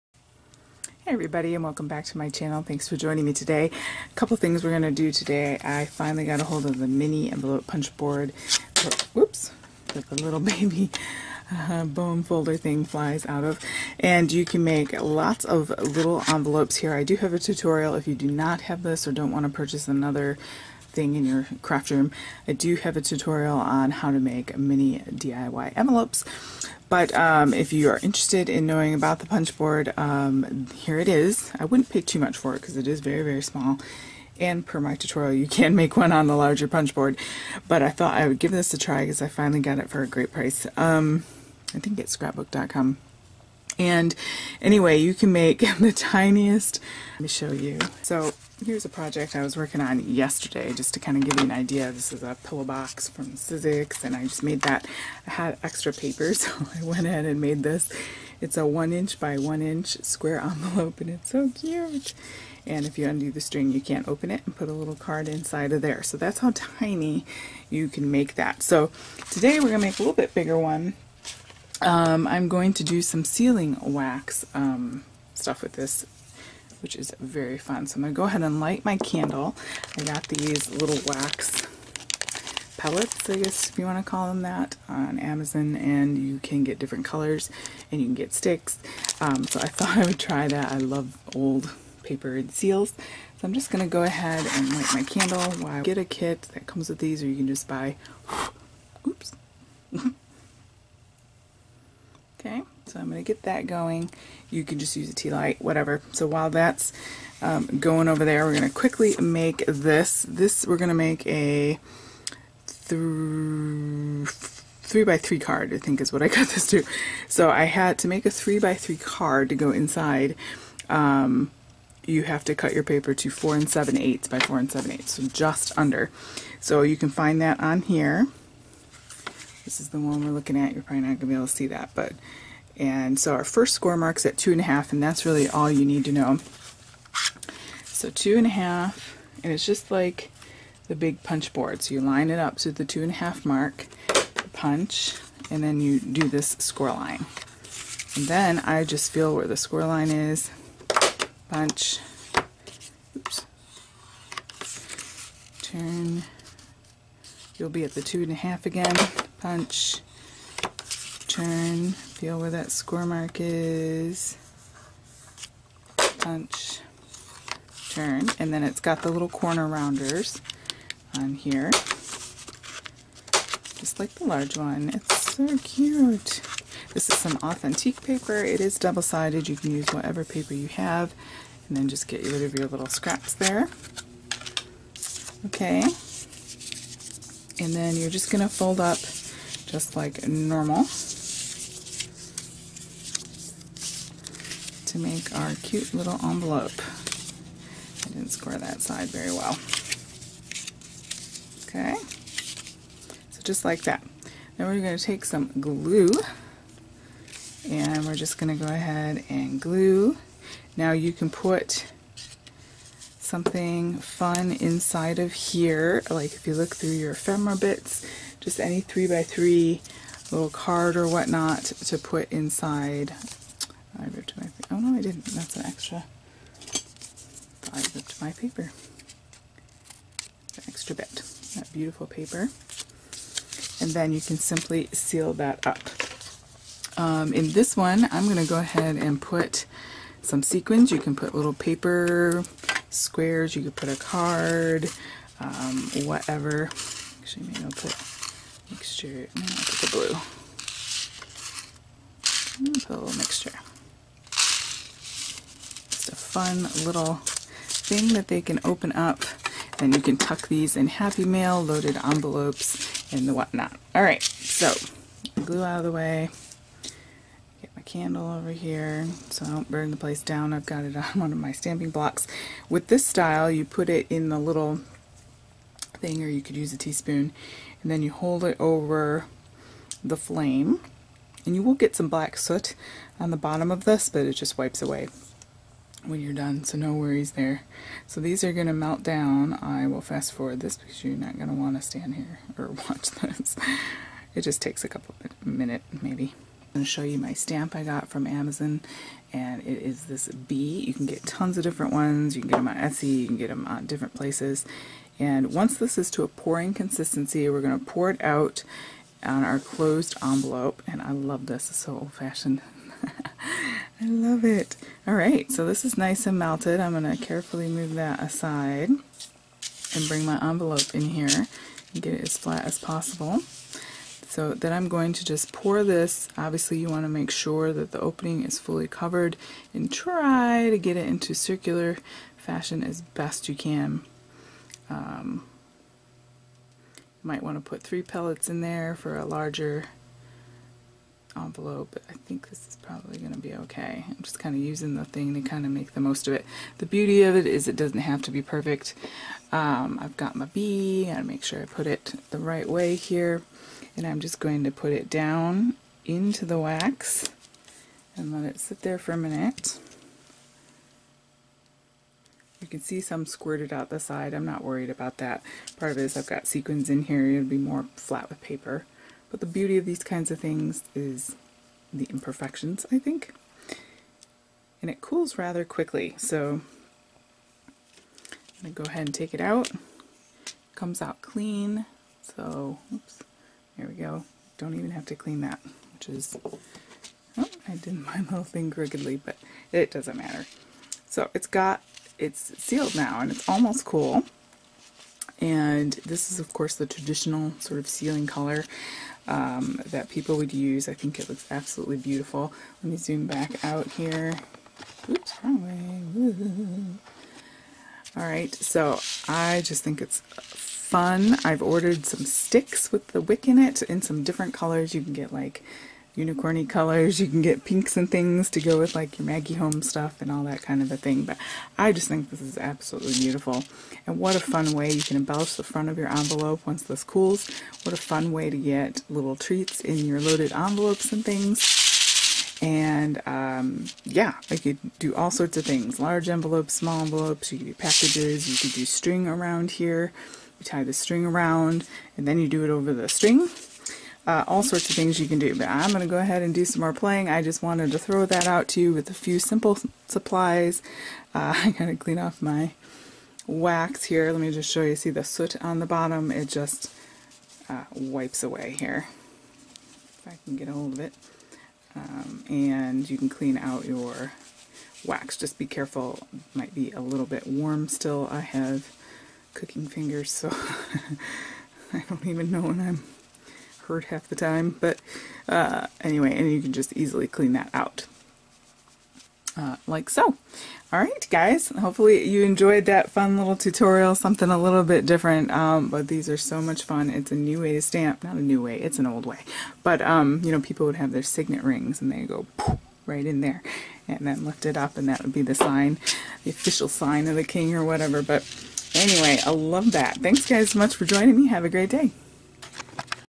WRMK Mini Envelope Punch Board & Wax Sealing Tutorial
I've discovered wax sealing! It is so easy and adds so much to an envelope. I'm also using the WRMK Mini Envelope Punch Board.
MY BOOKS &JOURNALS